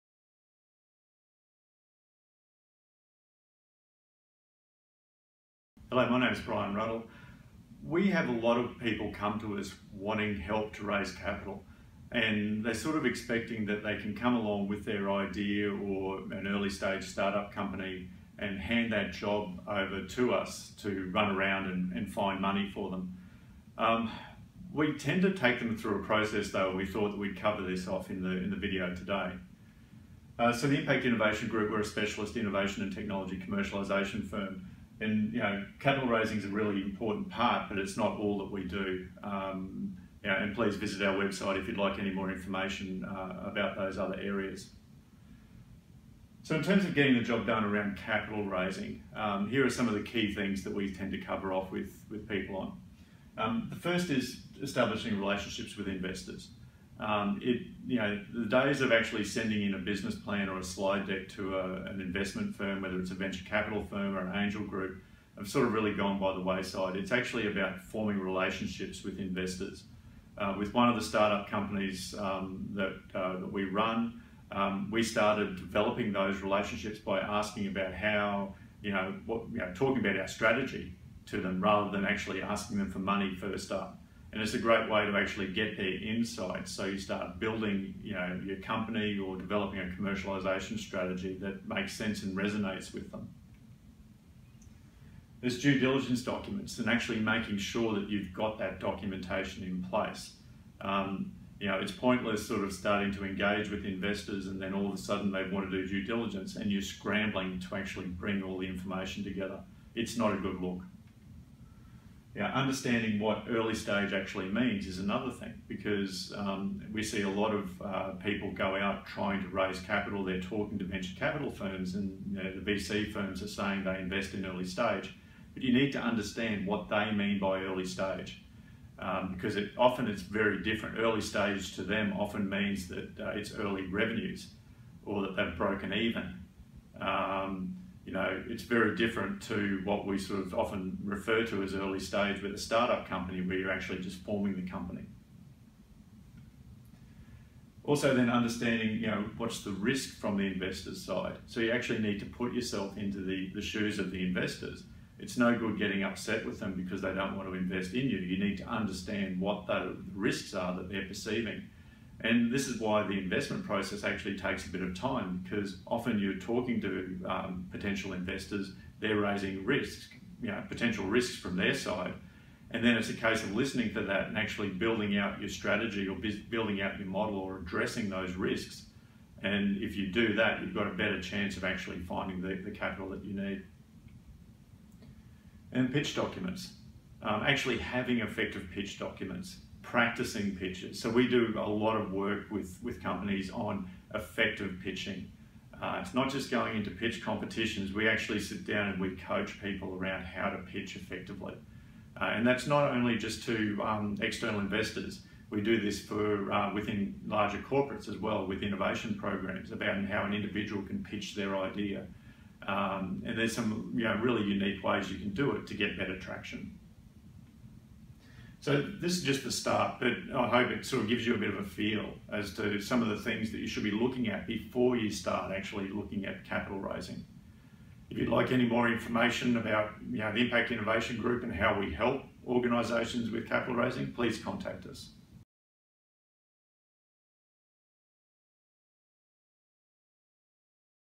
Things to do before capital raising
The activities to focus on before talking to investors.